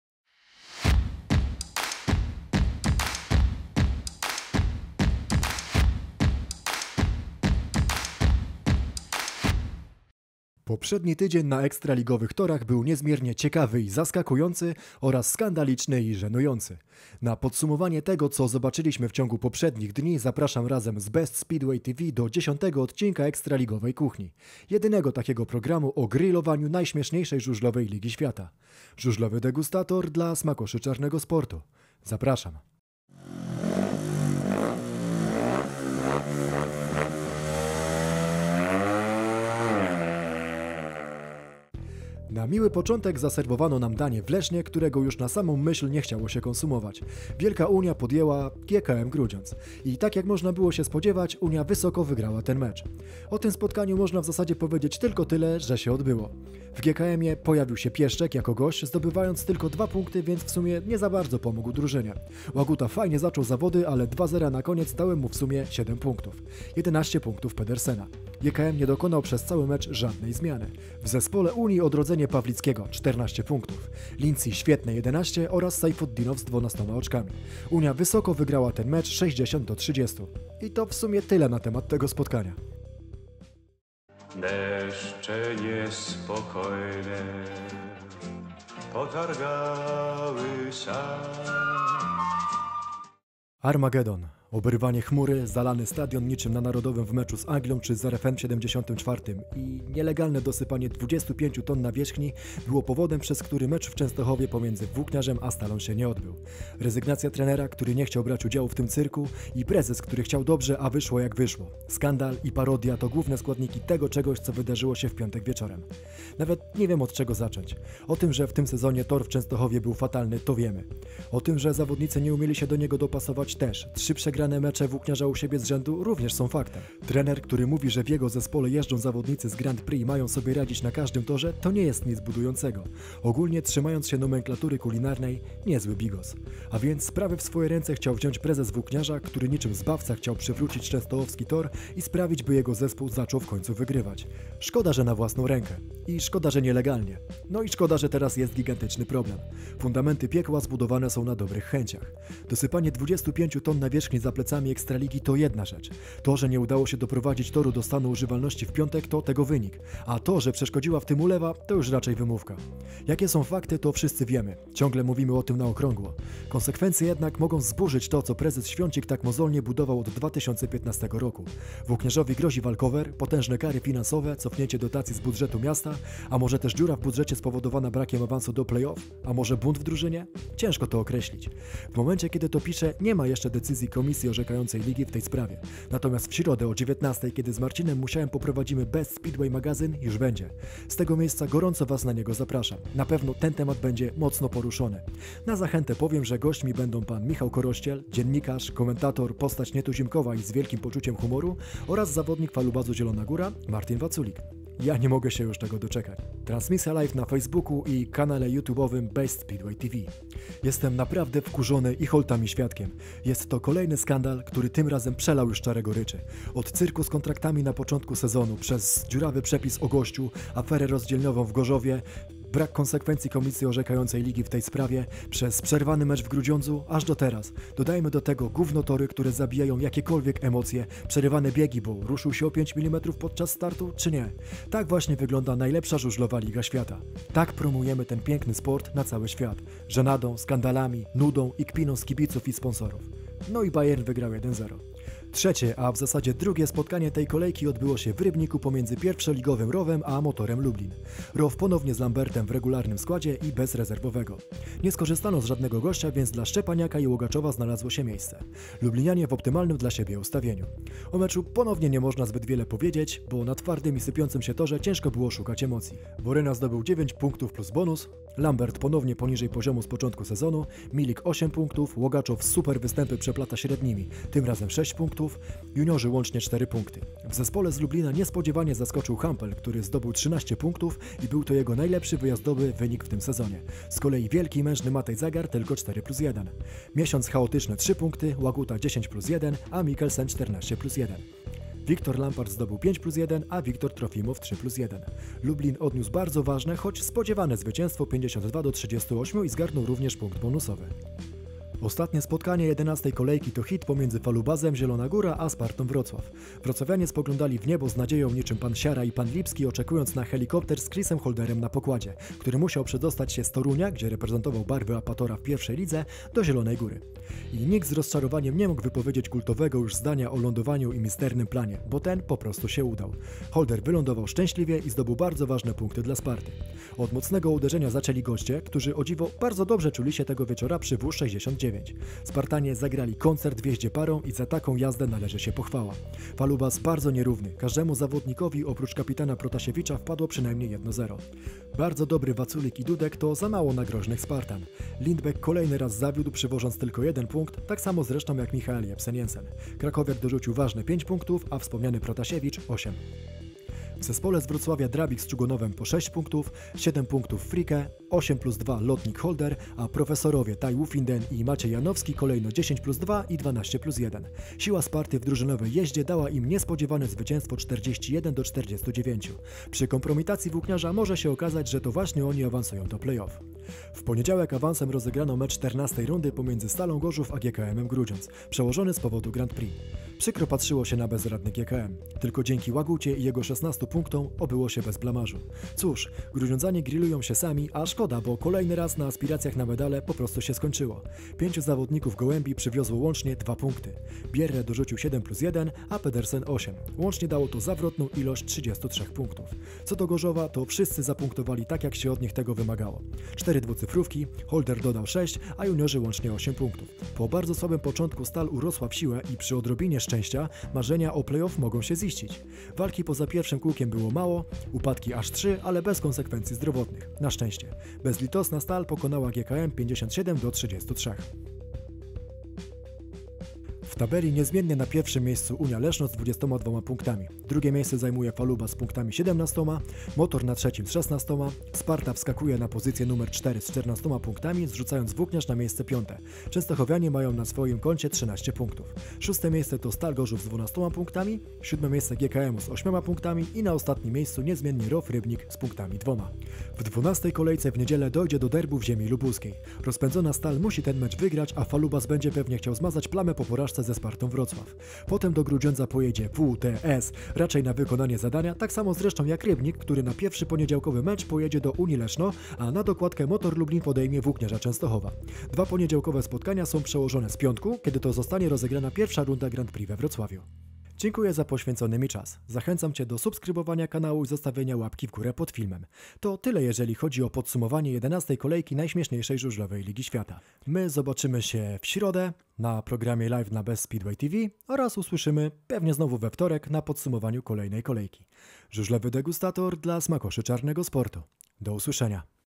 KOMPROMITACJA w Częstochowie, niespodzianka w Zielonej Górze / Ekstraligowa Kuchnia #010
Krótkie podsumowanie 11 kolejki PGE Ekstraligi żużlowej.

#011 Ekstraligowa Kuchnia

Narracja: Żużlowy Degustator

Audio: Żużlowy Degustator

Video: Żużlowy Degustator